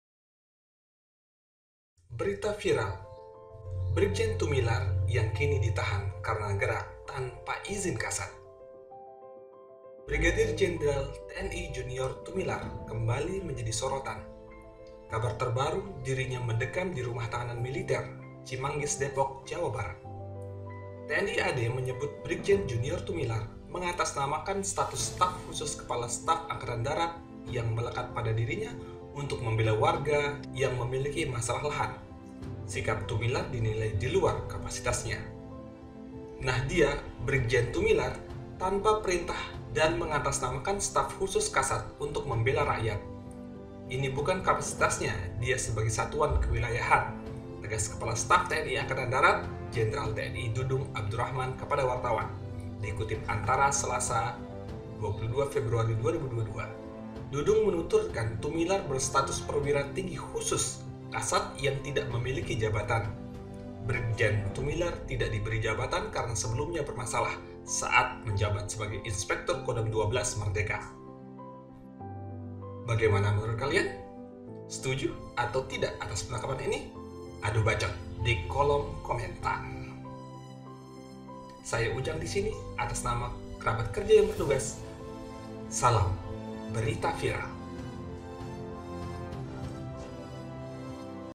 Berita viral ... Brigjen Tumilaar Kini Ditahan
Brigadir Jenderal (Brigjen) TNI JuniorTumilaar kembali menjadi sorotan. Kabar terbaru, dirinya mendekam di Rumah Tahanan Militer (RTM), Cimanggis, Depok, Jawa Barat (Jabar).
TNI AD menyebut Brigjen Junior Tumilaar mengatasnamakan status staf khusus Kepala Staf Angkatan Darat (KSAD) yang melekat di dirinya, untuk membela warga yang memiliki masalah lahan. Sikap Tumilaar dinilai di luar kapasitasnya, menurut Dudung Abdurachman.

Selamat menyaksikan video Berita viral ... Brigjen Tumilaar Kini Ditahan Semoga bermanfaat

Salam